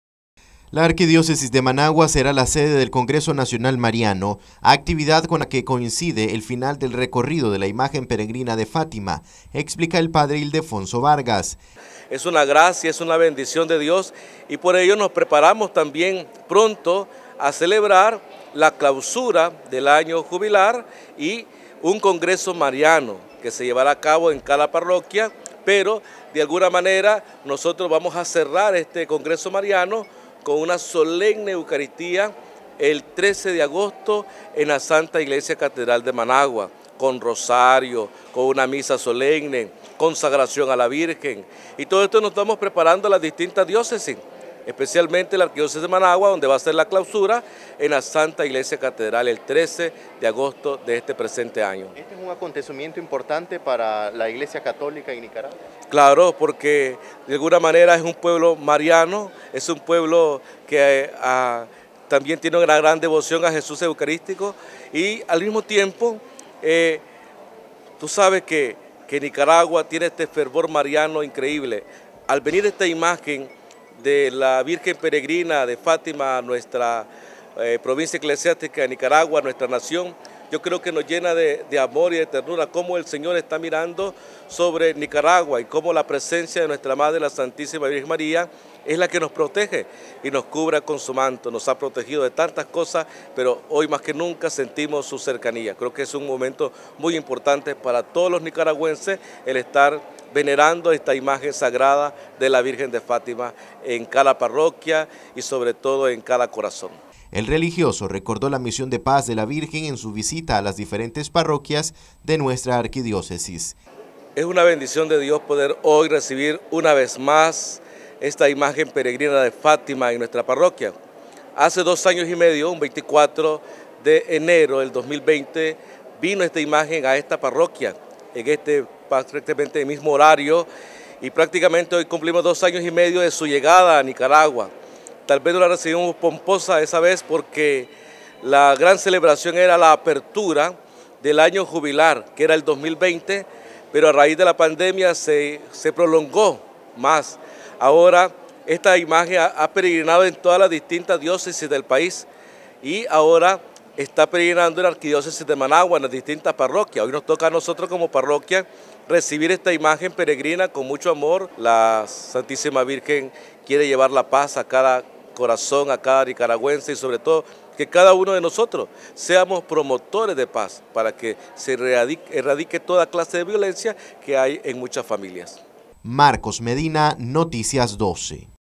Arquidiócesis de Managua será sede del congreso Nacional Mariano
Noticias12Nicaragua Arquidiócesis de Managua será sede del congreso Nacional Mariano.